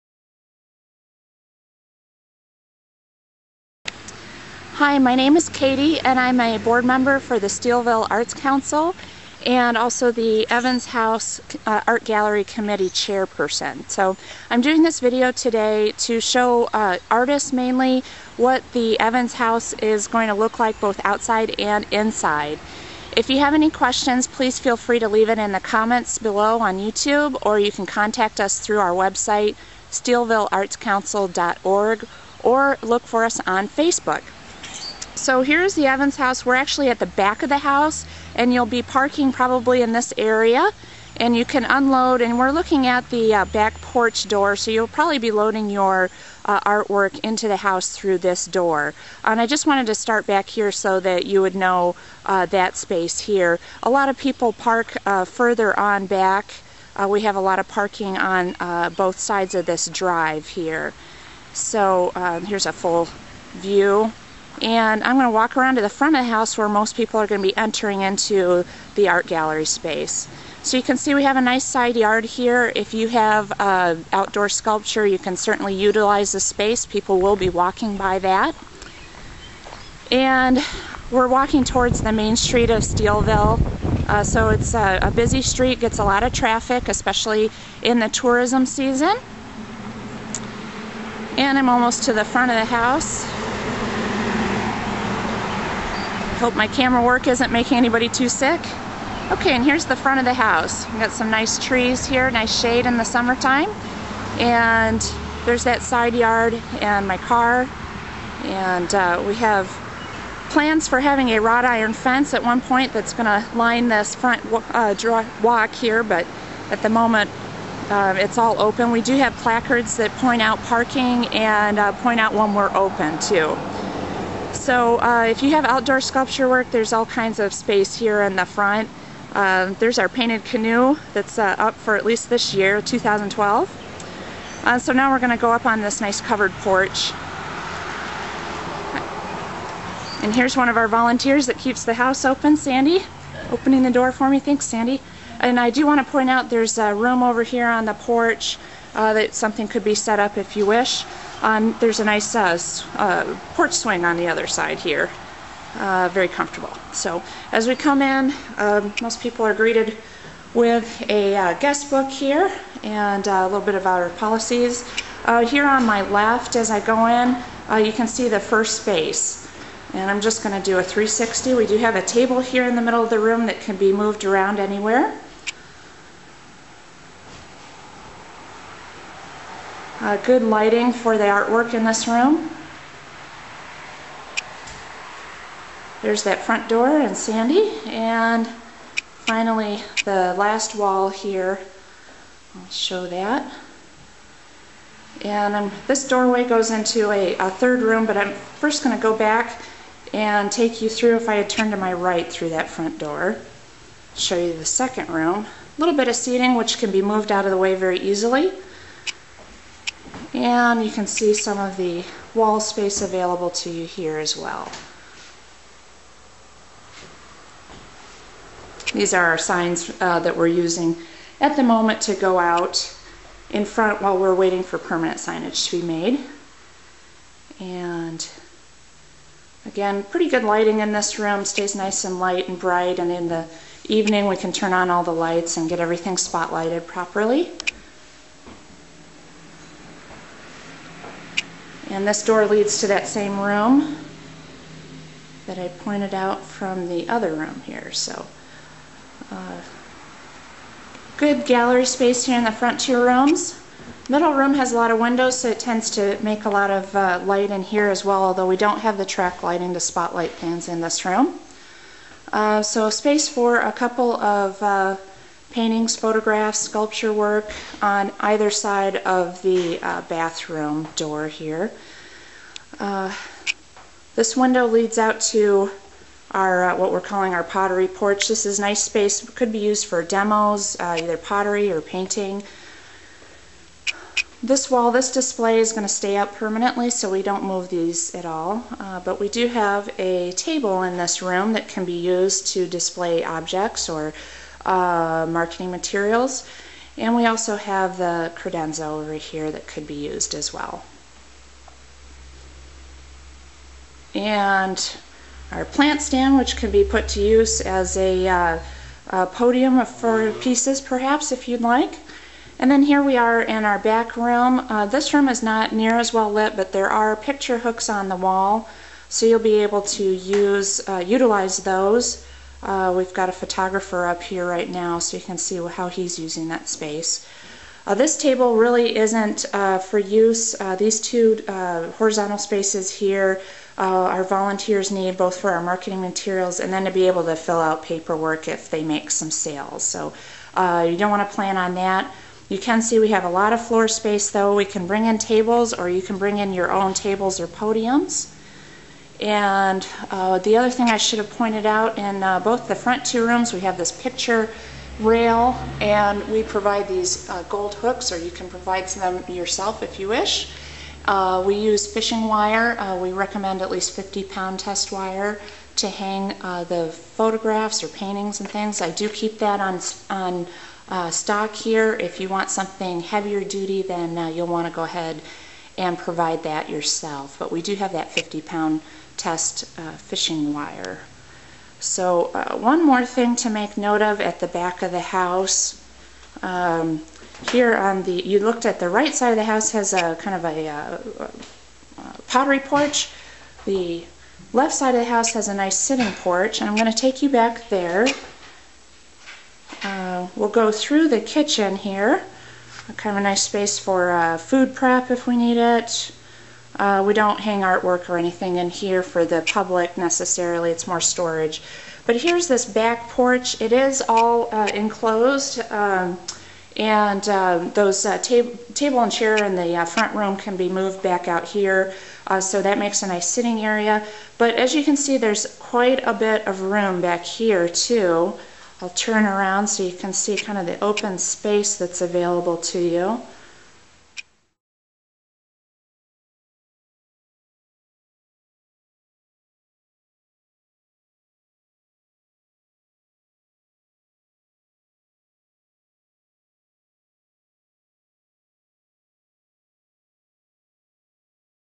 Evans House Tour.wmv
A short tour of the Evans House Art Gallery in Steelville aimed mainly for those artists who have not had a chance to visit the space yet.